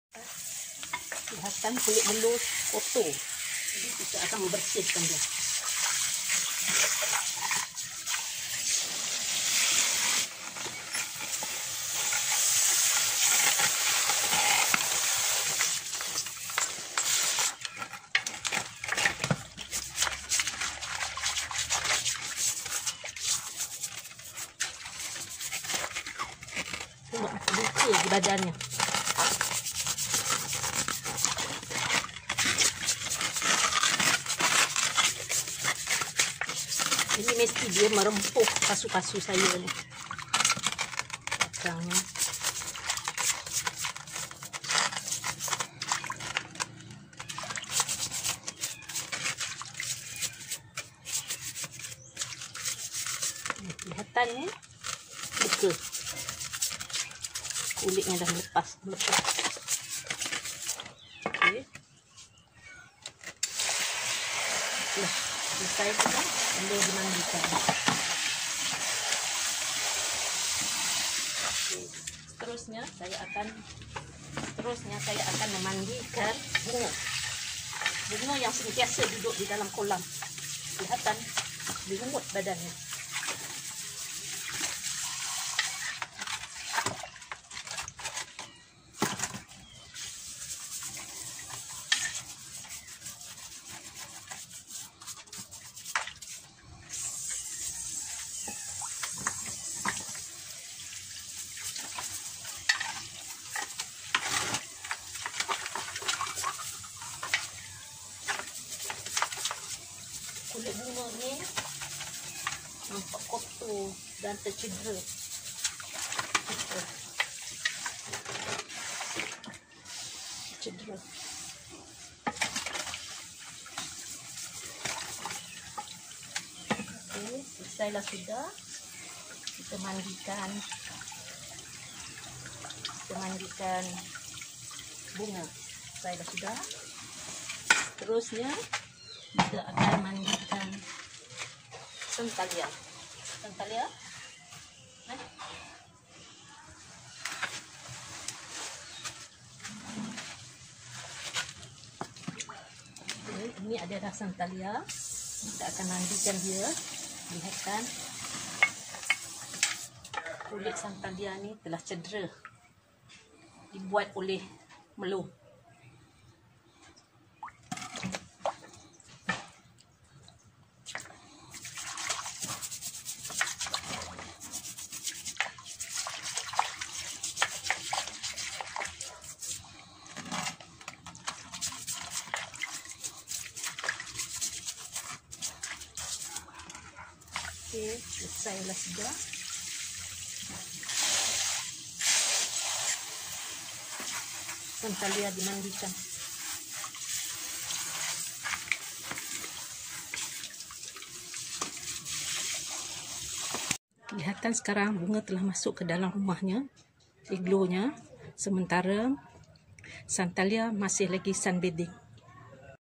Memandikan Kura-Kura Peliharaan
MemandikanKura-Kura Peliharaan ( Bathing
of My Turtle every 10 Days) 10 hari sekali.

3 ekor Kura-Kura Peliharaan Saya di kenali sebagai Melor, Bunga dan Santhalia.

Kura-Kura ini akan di gosok dengan Berus semasa di bersihkan keseluruhan anggotanya
dan disembur dengan air yang mengalir.

Selepas di Mandikan,Kura-Kura ini akan di Jemur seketika kerana Sinaran Matahari elok
untuk Kesihatan Kura-Kura ini

Semoga Cara-Cara  Memandikan Kura-Kura ini
dapat memberi manaafa kepada Kawan-
Kawan ya.

cikahchikchopchannel memandikan kurakurapeliharaan#bathingofmyturtlepet